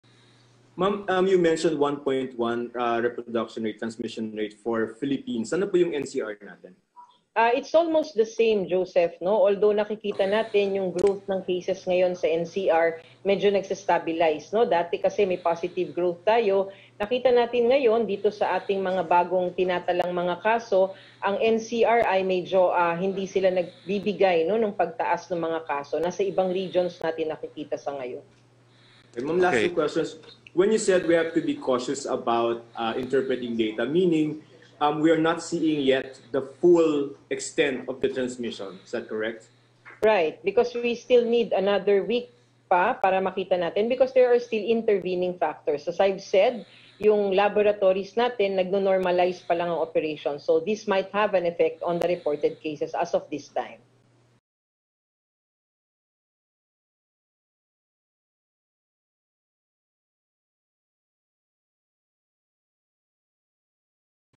No surge in COVID 19 cases in NCR for now, increase seen in other regions—DOH
Reported cases of coronavirus in Metro Manila have yet to show indications of a holiday surge, the Department of Health (DOH) said Tuesday, noting that an increase in infections was seen in other regions.